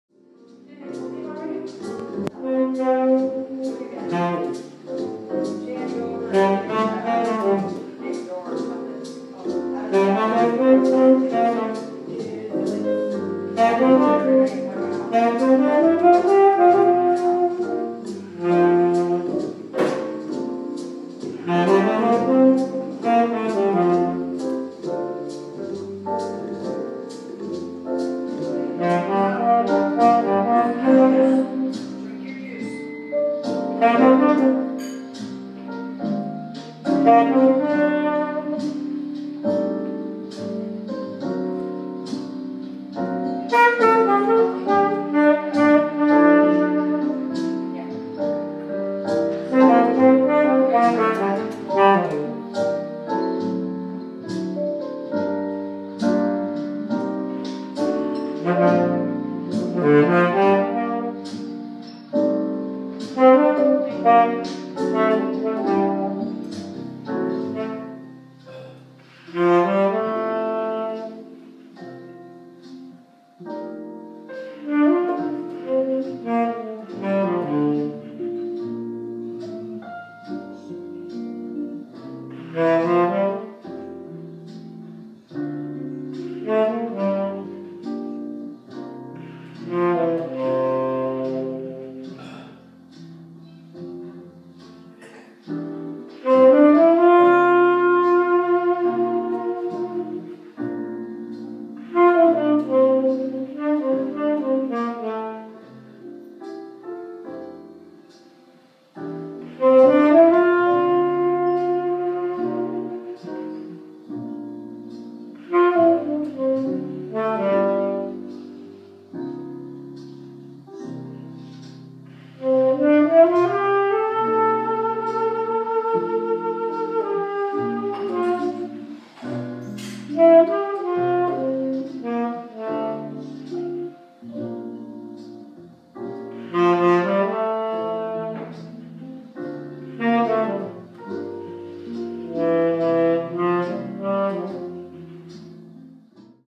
Tenor Sax at the Desert Sun House (Bradford Soland)
First time playing sax at for the folks at Desert Sun House in Tucson, AZ.  1/29/16.  Visit bradfordsoland.com for more information about music in Tucson care homes.